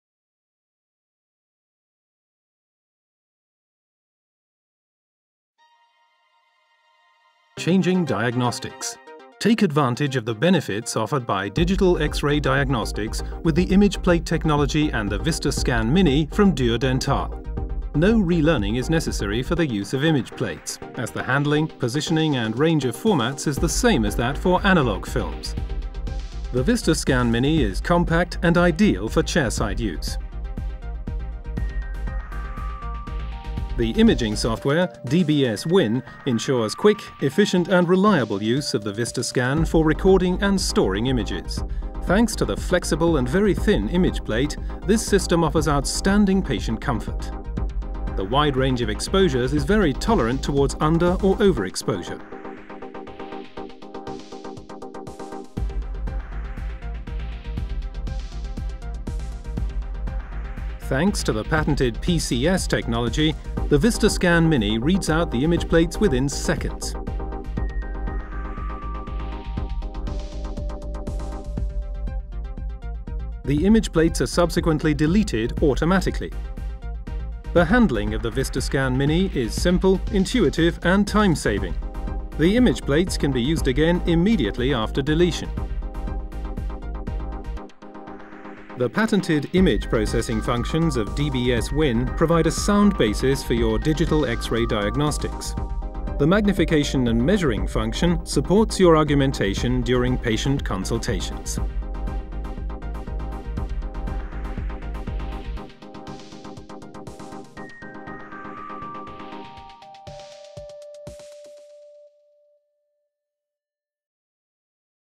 VistaScan Mini - Image Plate Size 2
The VistaScan Mini image plate scanner makes image plate diagnostics even faster for dentists. The compact device is particularly easy to use and requires a minimum of space -- so that it can be installed in the treatment room. The advantage: X-ray and scanning directly at the chairside with full flexibility in the image formats. The reusable VistaScan image plates are read out in top quality within seconds. There has never been a better time to change over to image plates.